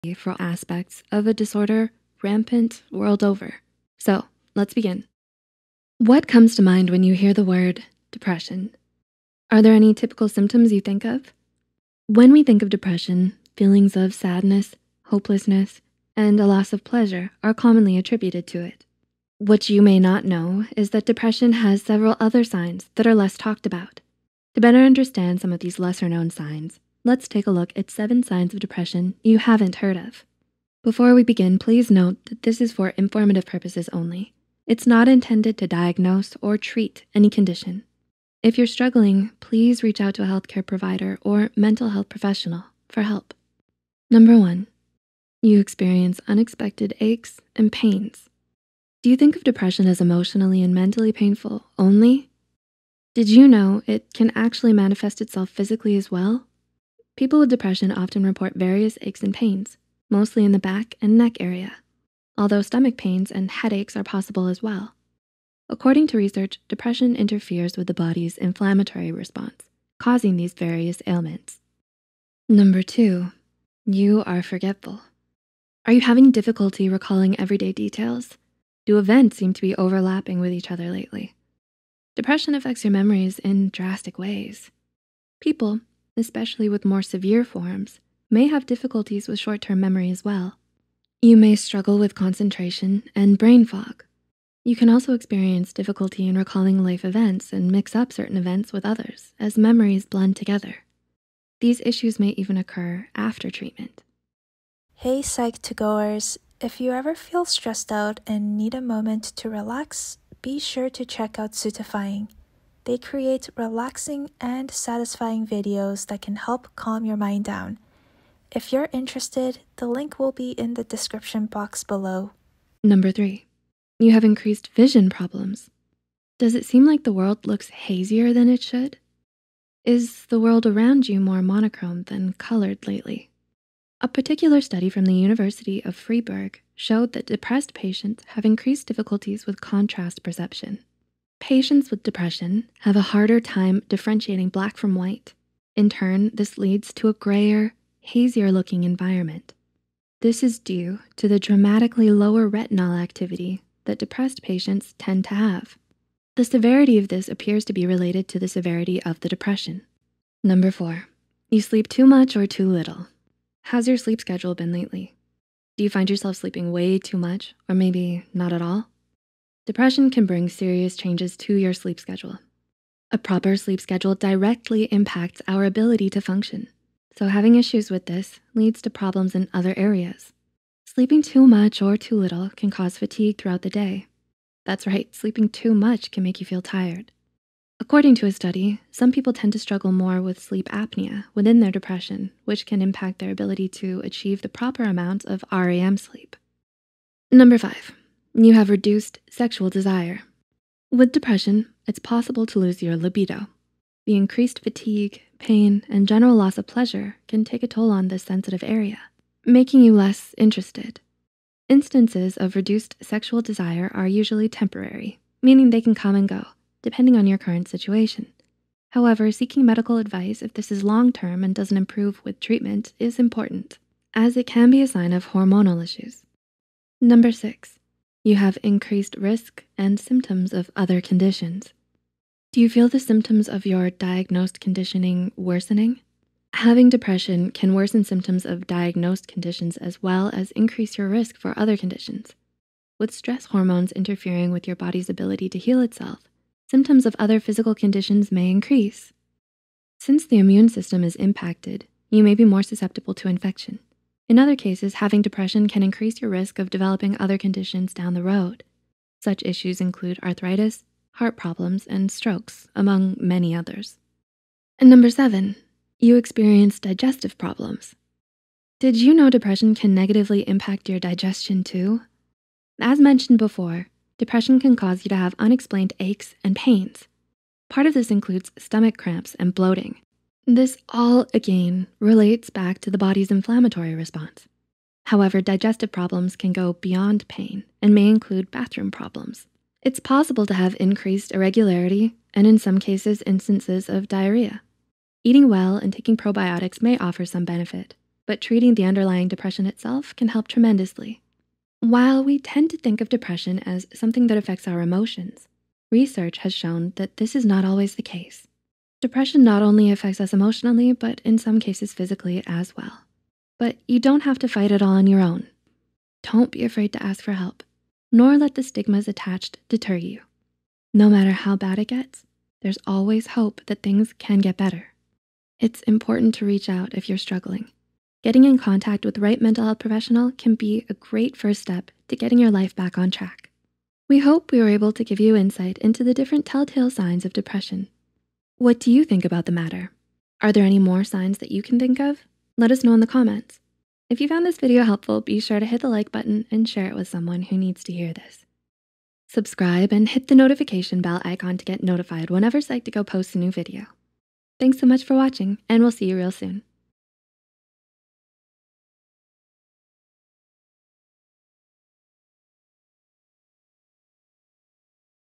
7 Signs of Depression You Haven't Heard
Are you experiencing some of these less known signs of depression? Even though these depression symptoms are not part of the DSM criteria, you should still consult a professional if you experience many of them.

Writer: Sid Thompson
Script Editor: Rida Batool
Script Manager: Kelly Soong
VO: Amanda Silvera
Animator: Aury
YouTube Manager: Cindy Cheong

References:
Cleveland Clinic. (2020, October 16). What You Should Know About the Relationship Between Oversleeping and Depression. Health Essentials from Cleveland Clinic.
Fraga, J. (2019, August 15). 7 Physical Symptoms That Prove Depression Is Not Just ‘In Your Head.’ Healthline.
Harvard Health Publishing. (2010, November). The Quirky Brain: How depression may alter visual perception. Harvard Health.
Higuera, V. (2020, February 11). Everything You Want to Know About Depression. Healthline.
Payne, L. (2020). Low Sex Drive — Could It Be a Sign of Depression? HopkinsMedicine.
Rethink. (2020). What are the signs and symptoms of depression. Rethink.Org.
Stuart, A. (2009, July 8). How Depression Affects Your Body. WebMD.
Villines, Z. (2020, July 22). Are depression and memory loss connected? MedicalNewsToday.